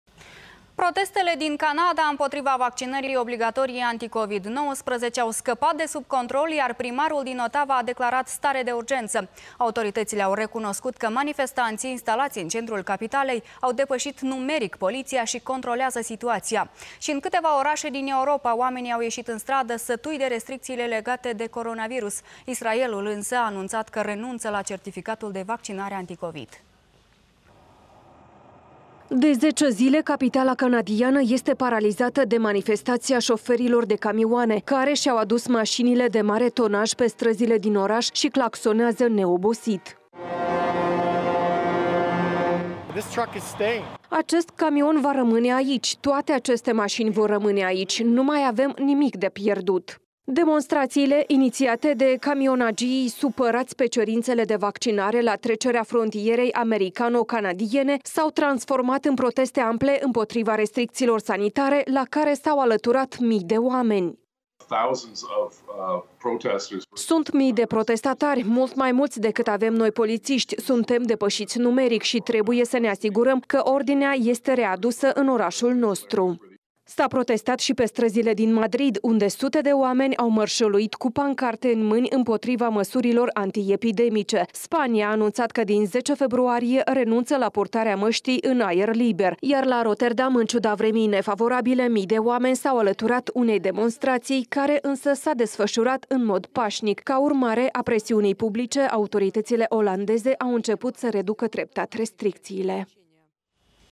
Protestele din Canada au scăpat de sub control. Primarul din Ottawa a declarat stare de urgenţă
Protestele din Canada împotriva vaccinării obligatorii anti-COVID au scăpat de sub control, iar primarul din Ottawa a declarat stare de urgenţă. Autorităţile au recunoscut că manifestanții, instalaţi în centrul capitalei, au depăşit numeric poliţia şi controlează situaţia. Şi în cîteva oraşe din Europa oamenii au ieşit în stradă, sătui de restricţiile legate de coronavirus. Israelul însă a anunţat că  renunță la certificatul de vaccinare anti-COVID